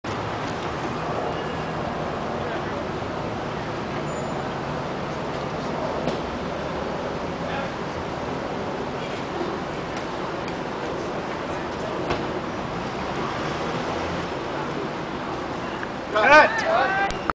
movie filming
Brad Pitt outside of the I-House at the "Burn after reading" movie shooting
9/24/07